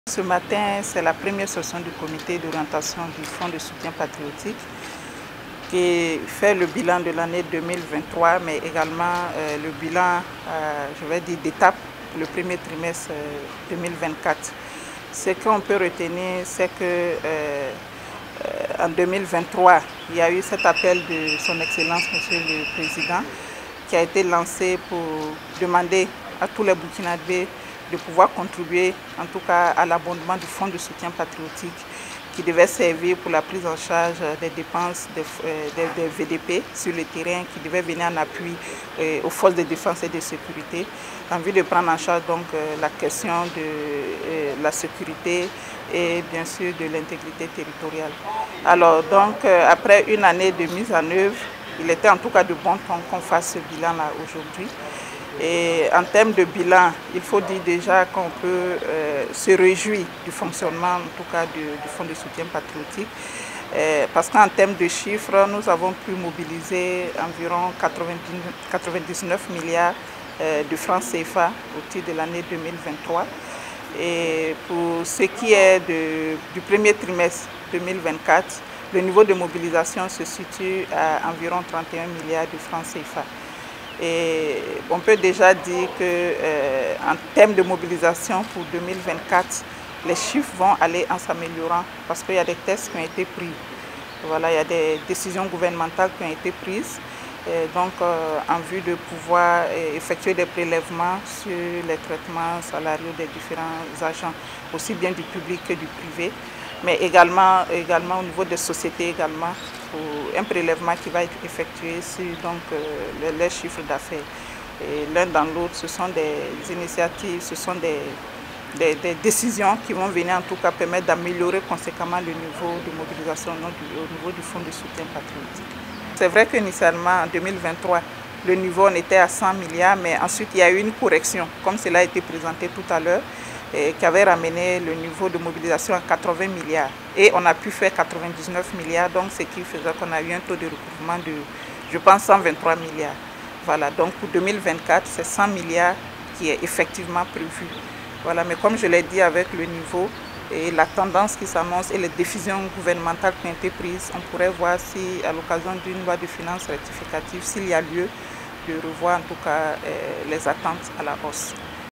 1ère session de l'année 2024 du Conseil d'orientation du fonds de soutien patriotique
Le Conseil d'orientation du fonds de soutien patriotique a tenu sa première session de l'année 2024, ce mardi 26 mars à Ouagadougou sous la présidence de Dr Apollinaire Joachimson KYELEM de TAMBELA., Chef du gouvernement burkinabè.

Cette session a porté sur l’examen et l’adoption du bilan de mise en œuvre du FSP en 2023, le rapport de gestion 2024 et le projet d’affectation de ressources aux bénéficiaires.